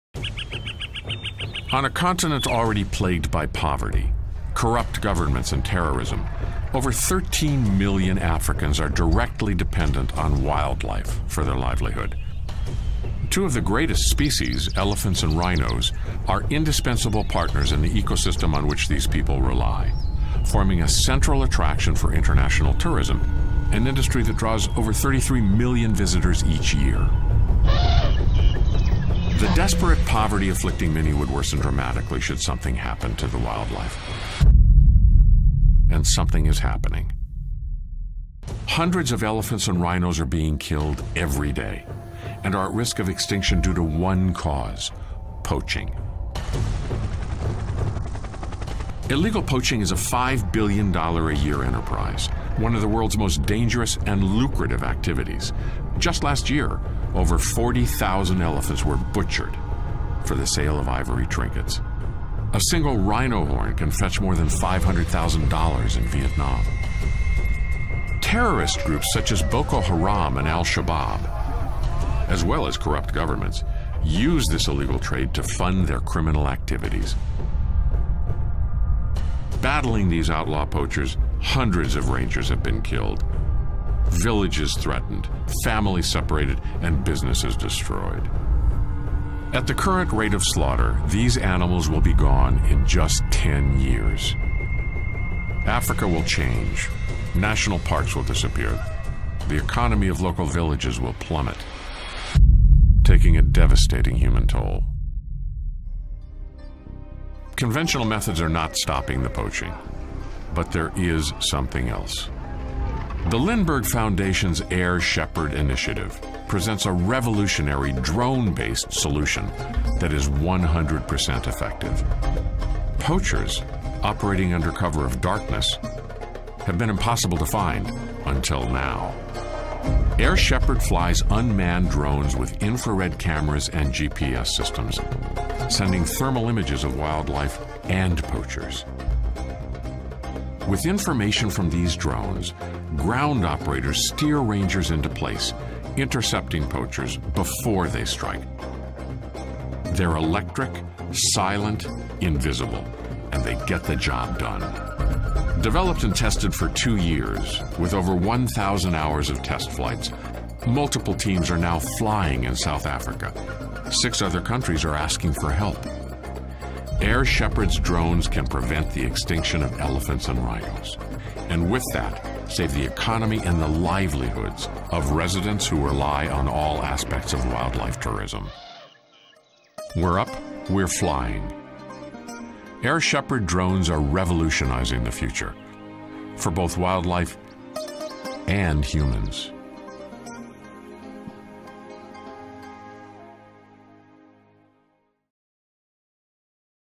Air Shepherd Initiative
See how the Lindbergh Foundation's Air Shepherd Initiative use unmanned drones to help fight poachers in Africa.